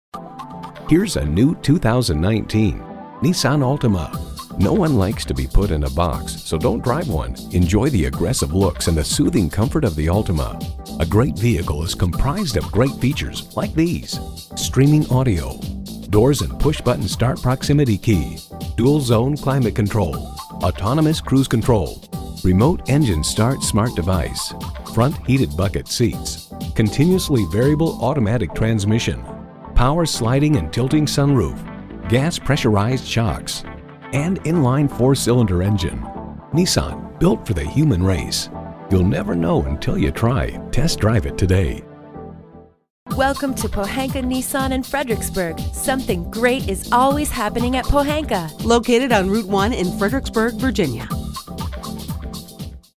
New 2019 Nissan Altima Richmond VA Fredericksburg, VA NKC179744 - SOLD
Year: 2019
Make: Nissan
Model: Altima
Trim: 2.5 SV
Engine: 2.5L 4-Cylinder
Transmission: CVT with Xtronic
Color: Black
Interior: Charcoal
Stock #: NKC179744
VIN: 1N4BL4DV2KC179744
Super Black 2019 Nissan Altima 2.5 SV FWD CVT with Xtronic 2.5L 4-Cylinder CVT with Xtronic.brbrRecent Arrival! 28/39 City/Highway MPG

Address:
5200 Jefferson Davis Hwy